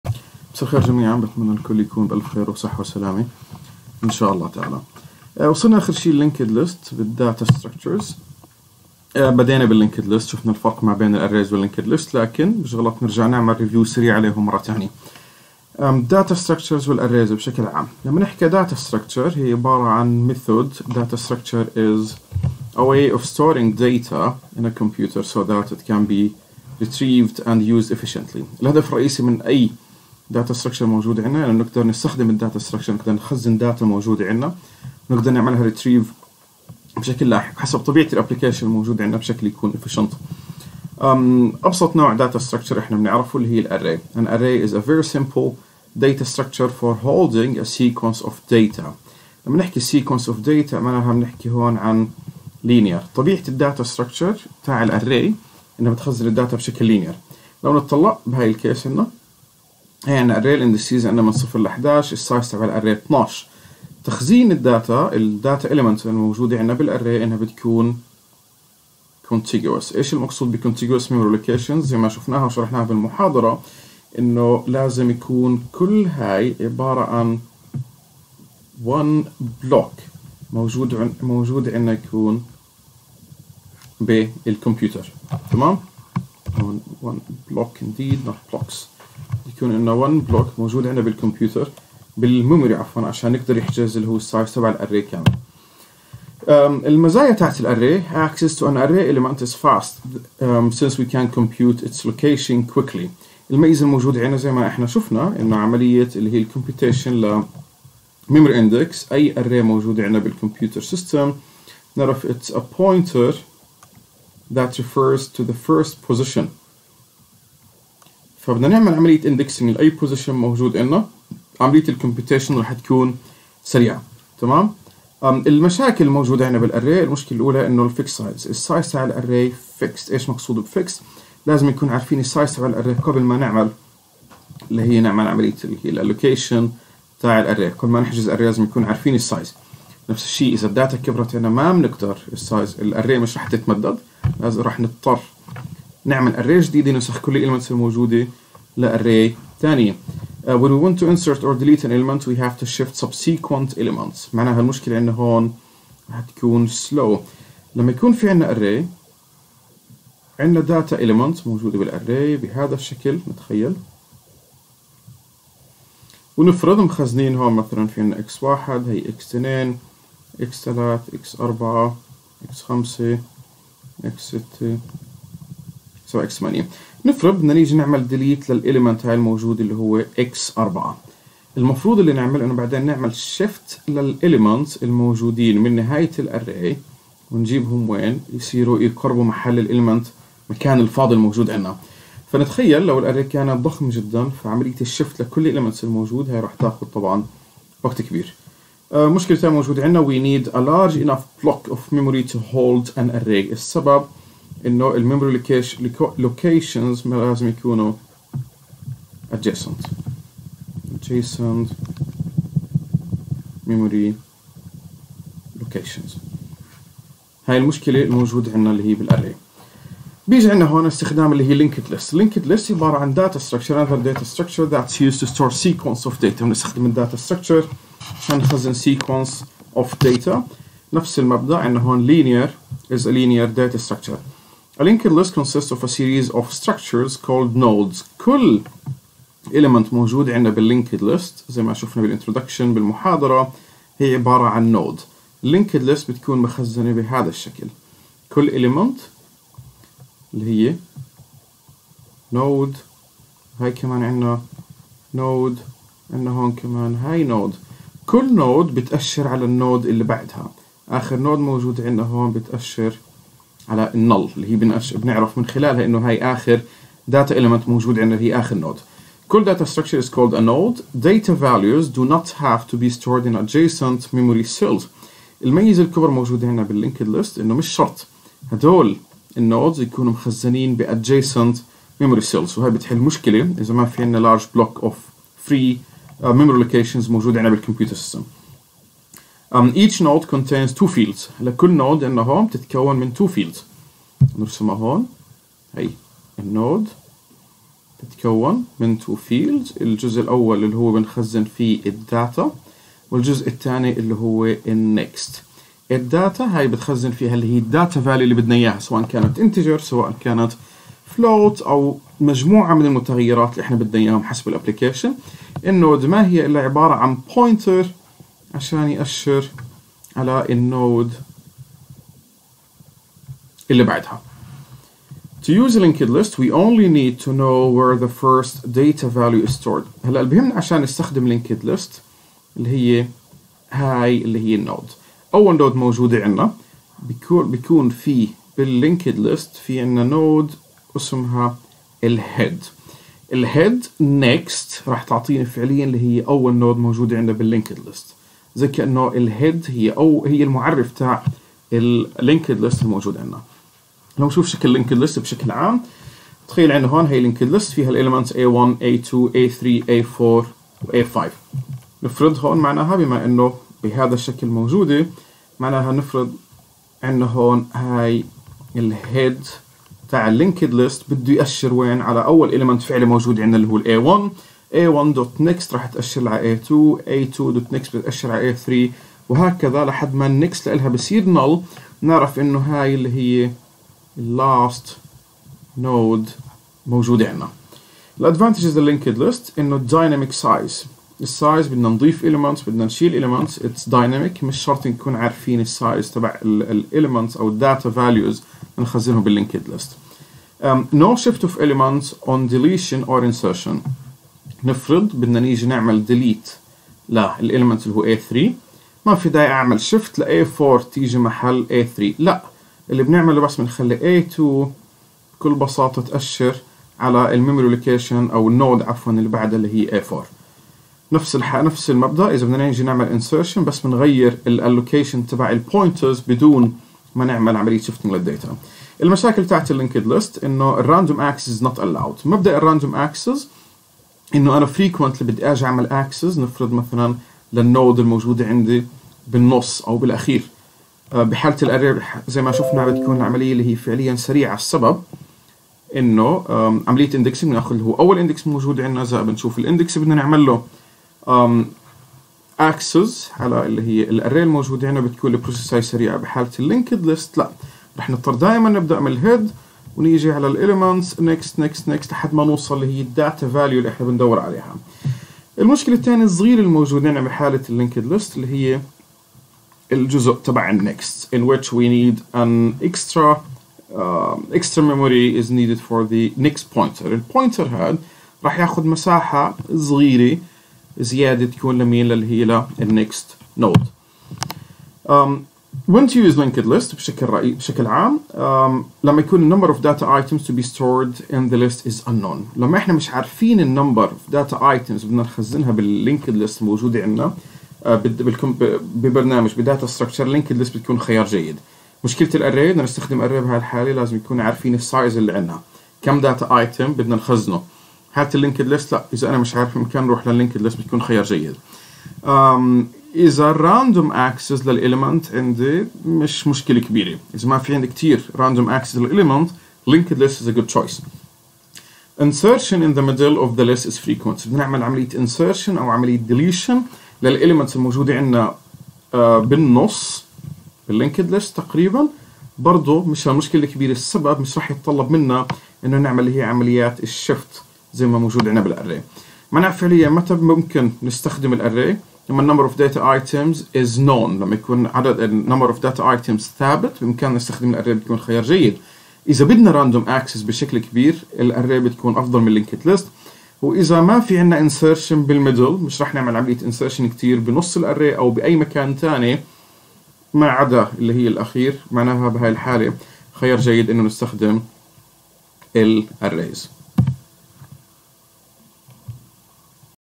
COMP2321 - Linked List Lecture 1 Dr. Radi Jarrar
COMP2321 - Data structures. Linked List Introduction.

Textbook: Weiss, Mark Allen. Data Structures and Algorithm Analysis in C: For Anna University, 2/e. Pearson Education India, 2002.